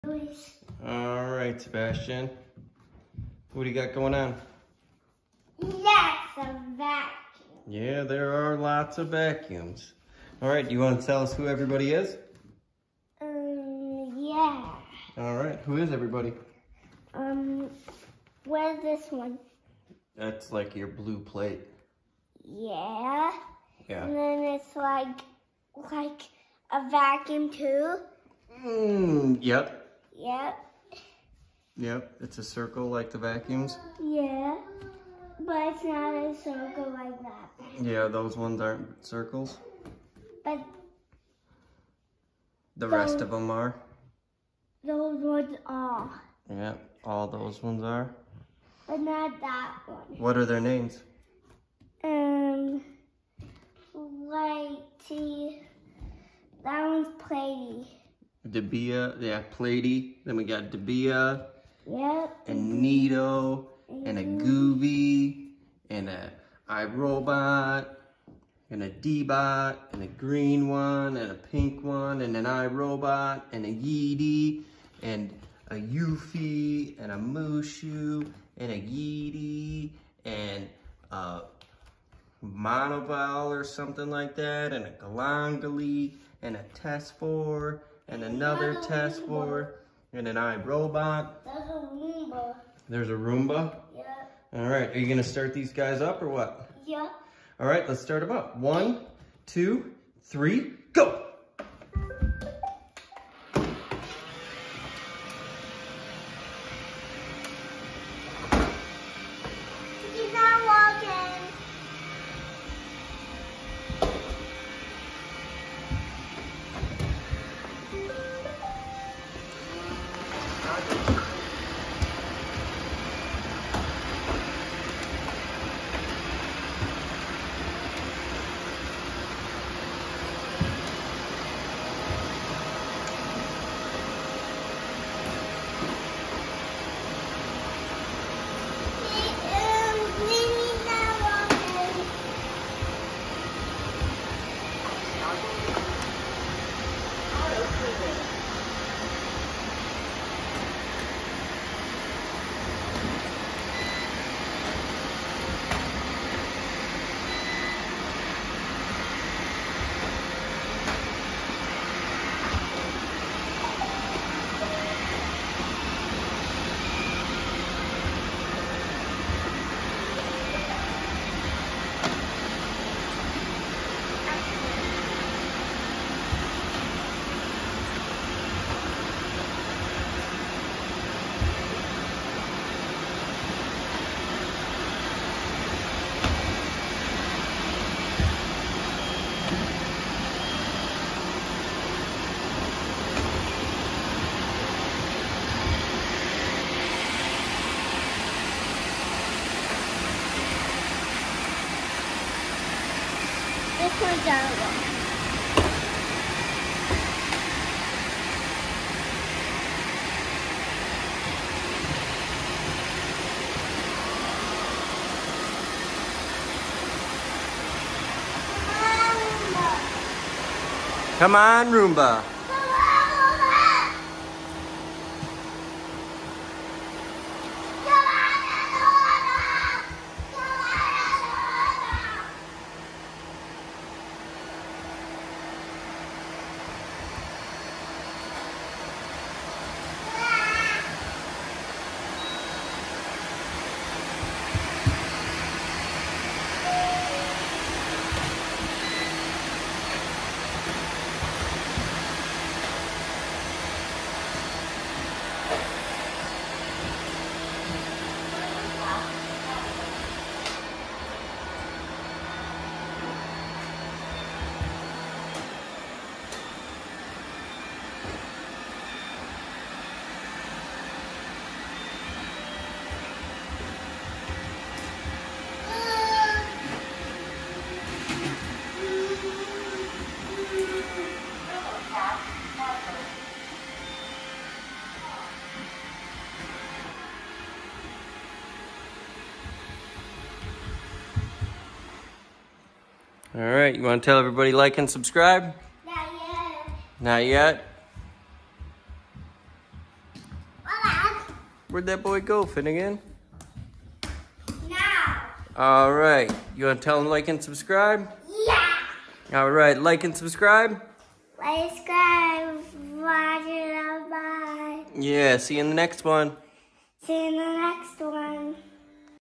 What is the best robot vacuum?? iRobot, yeedi, eufy, dibea, tesvor, neato, pure clean, golongele?
Robot vacuums running though the vacuum boy race!!! iRobot, roomba, yeedi, tesvor, dibea, ecovac, deebot, neato, pure clean, eufy, goovi, moneual, golongele